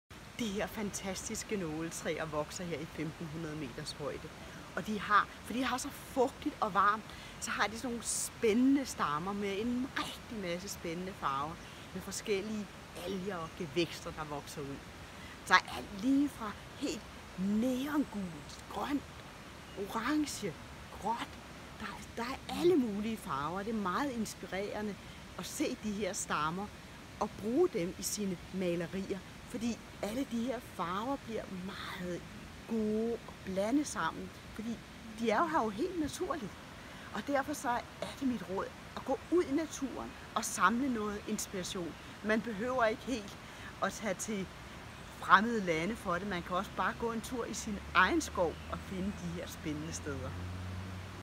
Tine Weppler: "Gå ud i naturen og find spændende detaljer"
På min tur på vulkanøen Reunion i det Indiske Ocean fandt jeg disse spændende cedertræer, hvor barken er meget fascinerende både i dens struktur og i farverne, der spænder fra neongult, over til grønt,  orange, gråt og brunt.

Selvfølgelig fotograferede jeg løs. Jeg gik helt tæt på og lagde mærke til alle detaljerne og har fået mange spændende motiver med mig hjem.

Her ser du nogle af mine fotos - og jeg vider også, hvordan jeg vel hjemme i mit galleri, bruger disse foto som inspiration til mine egne abstrakte malerier

Først sætter jeg farverne på min palette.Så er jeg gået i gang - jeg maler ikke ud fra et bestemt foto; men tager hele oplæevelsen med og giver udtryk for denne.

jeg har brugt. tape, som jeg sætter på for at lave streger i stil med barken. Det er sjovt at lege med - for tape gemmer noget, der ikke får maling - og på denne måde får dit abstrakte farverige maleri en spændende struktur.

Desuden undgår du,at farverne flyder helt sammen.

Så mit råd er, gå ud i naturen og find spændende detaljer.